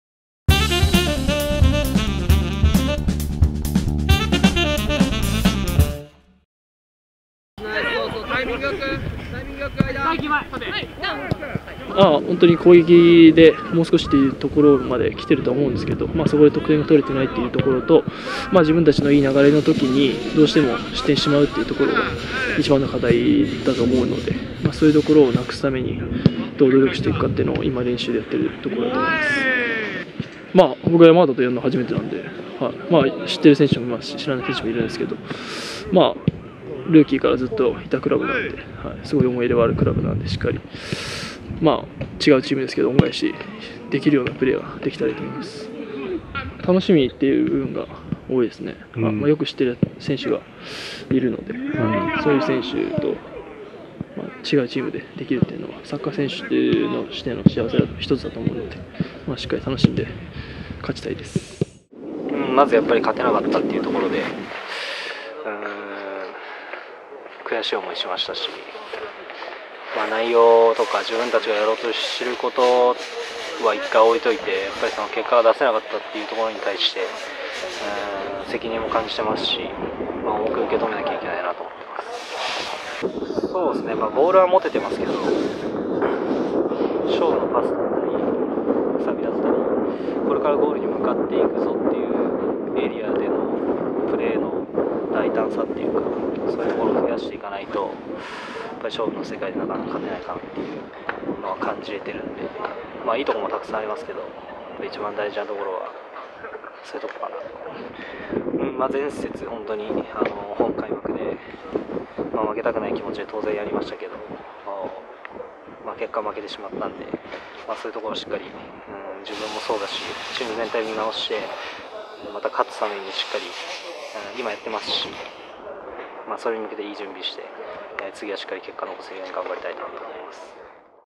徳島ヴォルティス2016 J2第3節・山形戦に臨む
熊本と対戦したホーム開幕戦は、後半にセットプレーから失点し、０−１で敗れた徳島ヴォルティス。開幕から悔しい敗戦が続いています。とはいえ、好機も多く作っており、次節のホーム山形戦での初勝利を期待したいところです。山形が古巣のDF石井秀典選手と、ボランチのMF岩尾憲選手にインタビューしました。